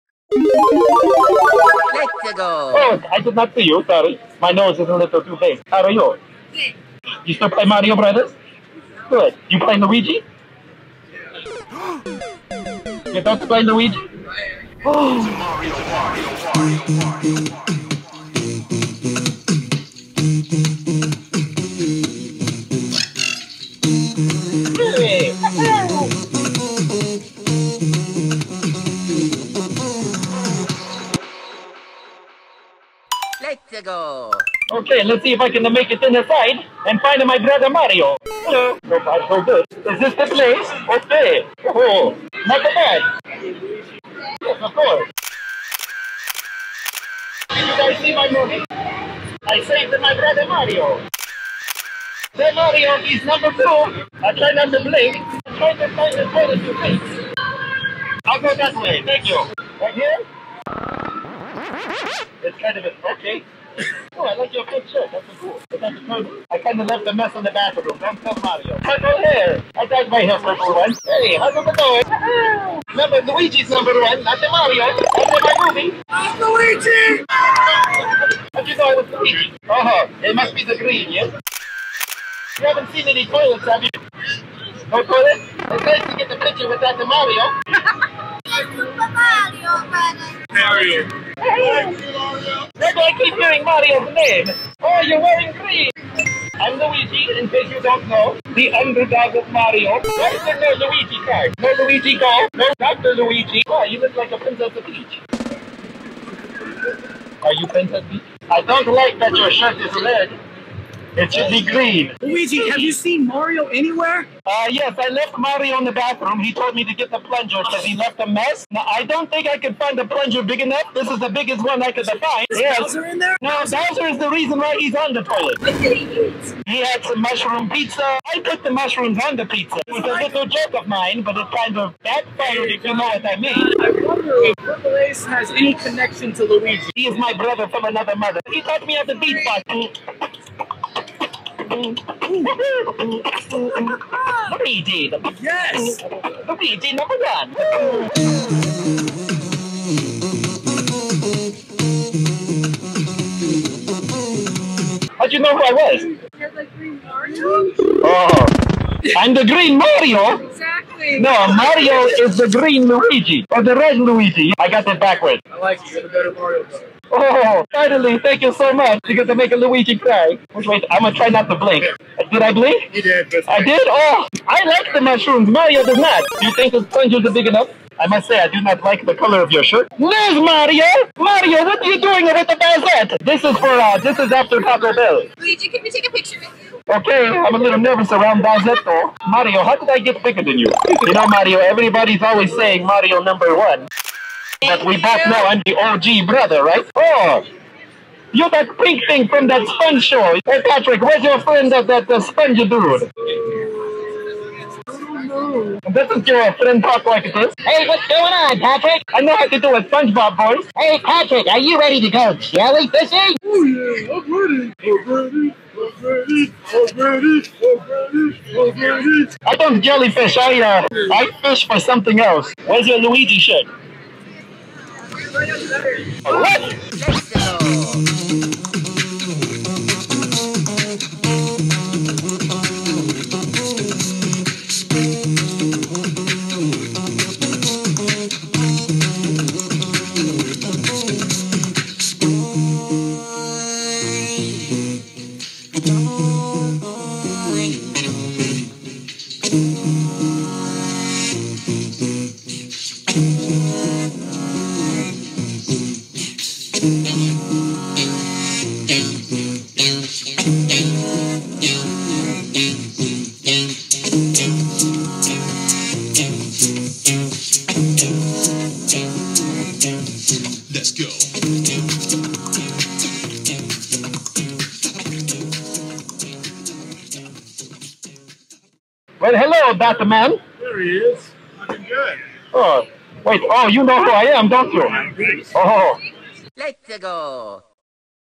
Luigi Visits Anime Las Vegas Convention
Luigi finally gets away from Mario in hopes that maybe he can take the spotlight for a little while! lol Huge shout out to my Bro Jay R Beatbox for the awesome videography and another huge shoutout to my bro Chazzy for the amazing edit!!

Thanks for your support, much love and respect to you all!! =D